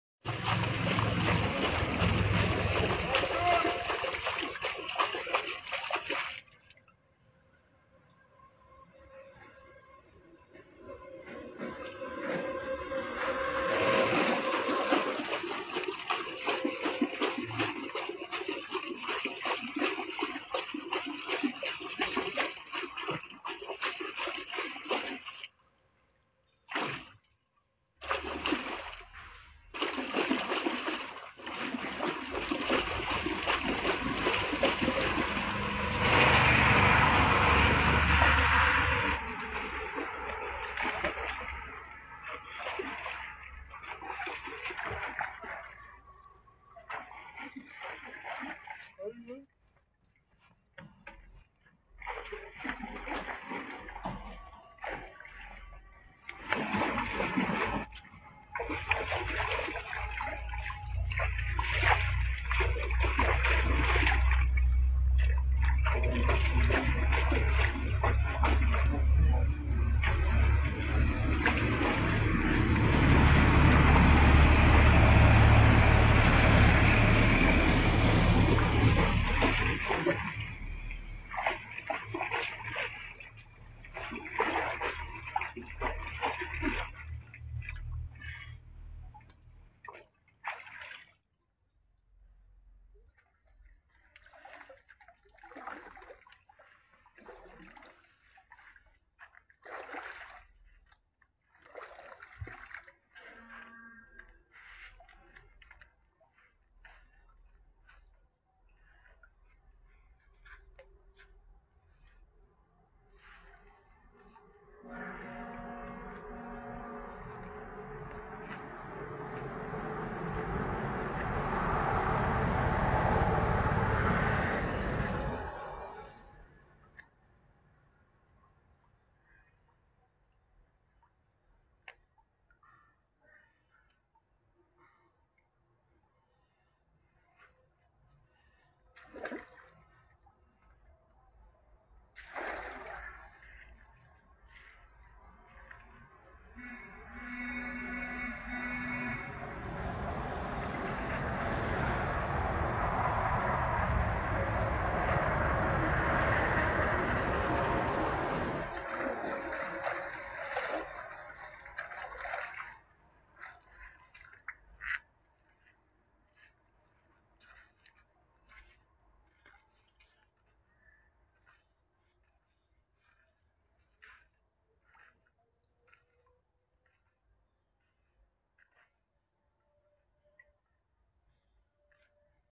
বাঁশের তৈরি হারলে মনে হয় বড় একটা মাছ ঢুকছে🫣🐬 finding video /fish🐟
Fishing Video ~ Grandpa showed extraordinary fishing talent in the field ~Amazing hook fishing

Various methods can be adopted for fishing in the field. But this method is one of them

My👉 YouTube👉. fishing bd24
My👉Facebook pages. fishing bd24